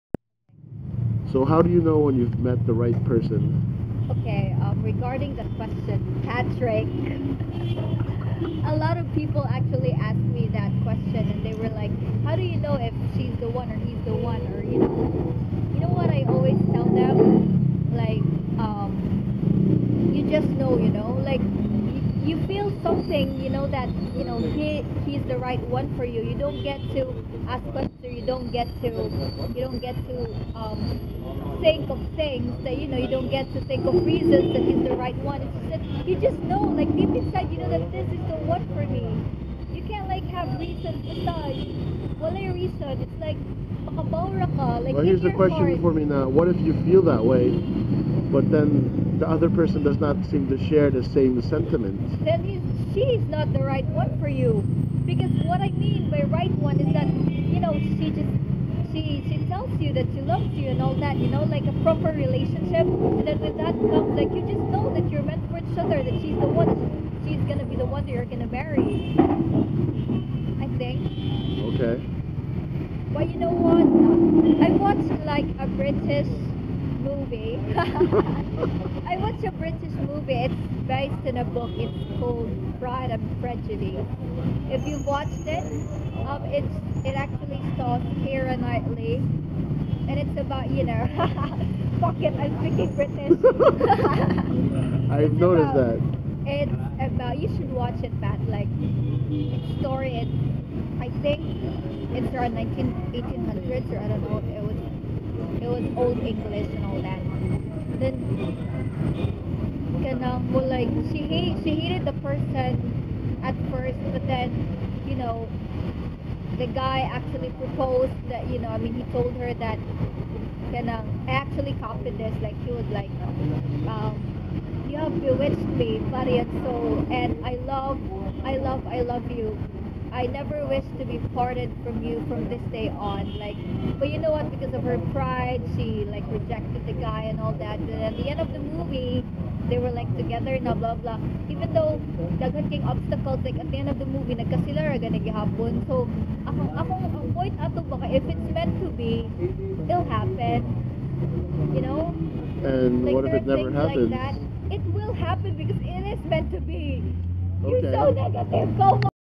How do you know when you've met the "right" person?
This is a continuation on my series about asking people their opinion on the usual relationship questions. :P